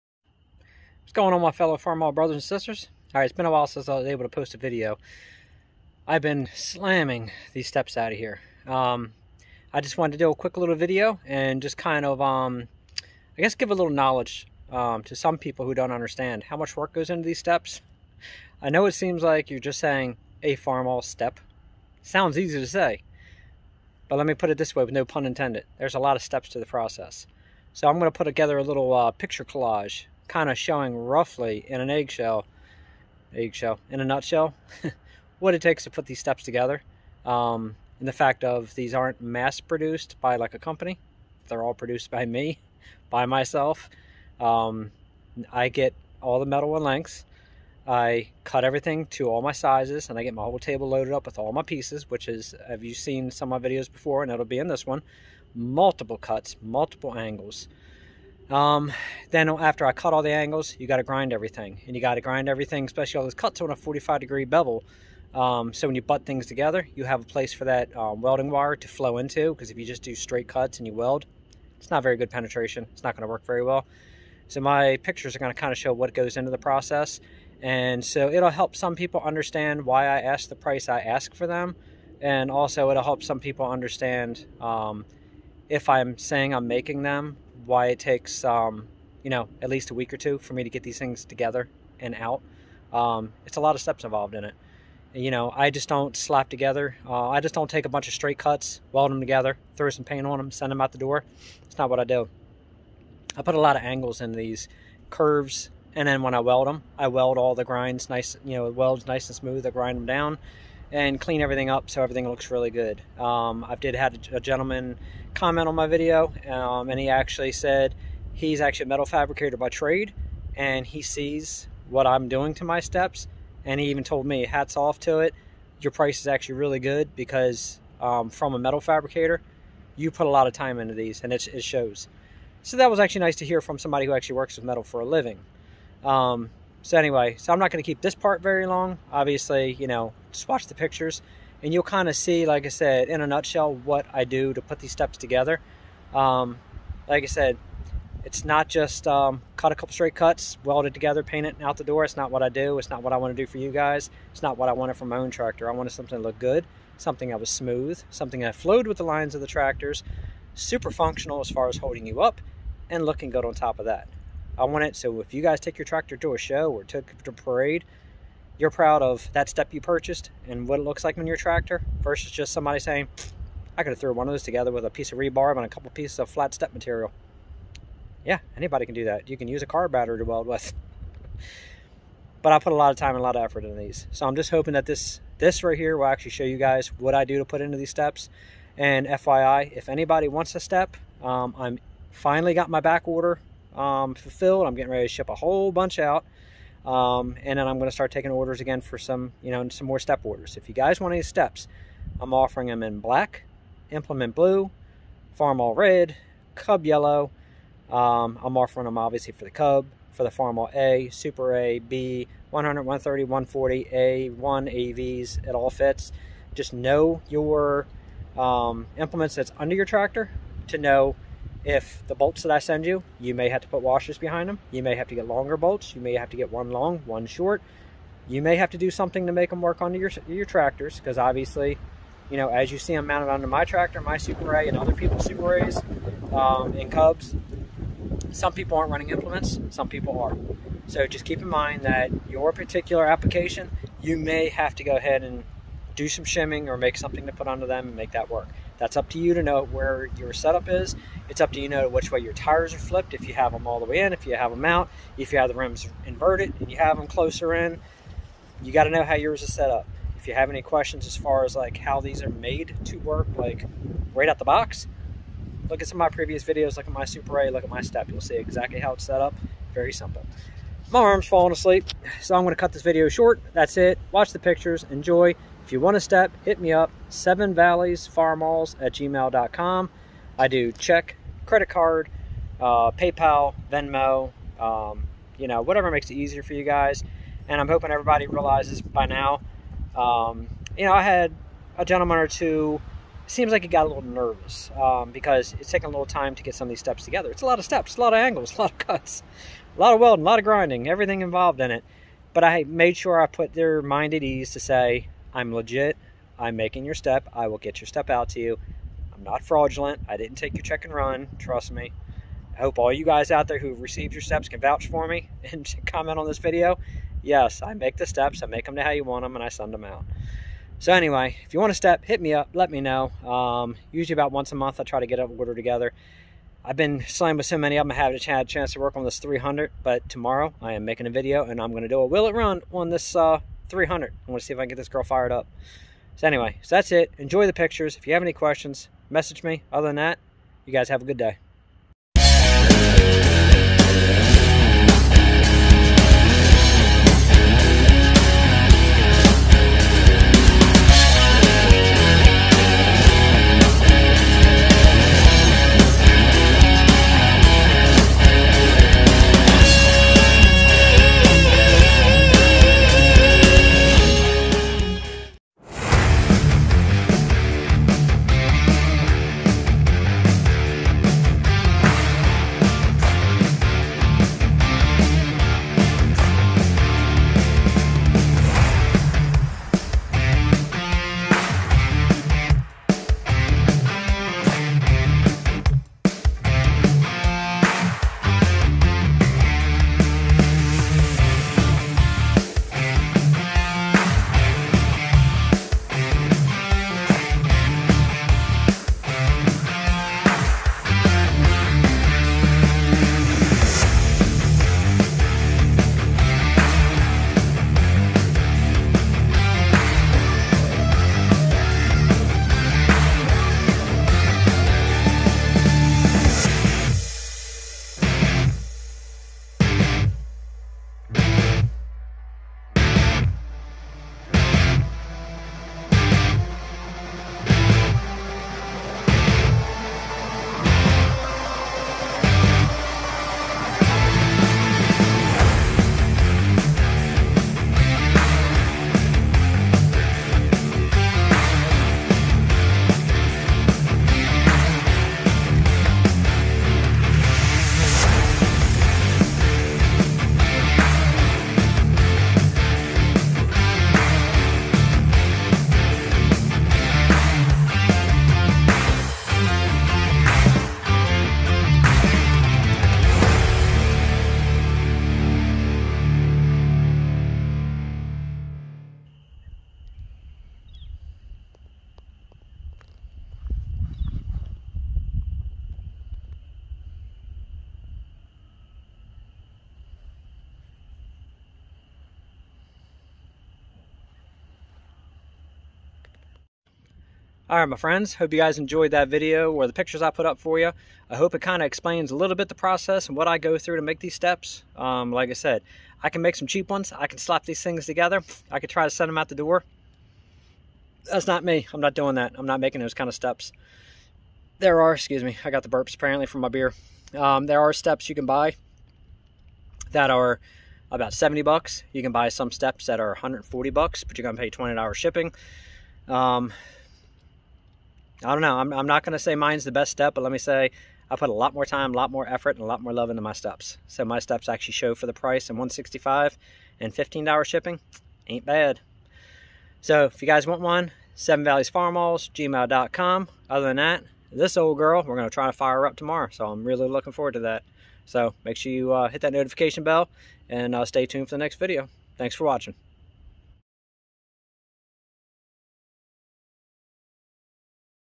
Farmall Building Step by Step
Just a little insight on what it takes me to build these beautiful steps!
Available for Cubs, Farmall A, SA, A1,AV,B,100,130,140 Tractors.
They come in Implement blue,  Cub yellow,  Farmall red or Black.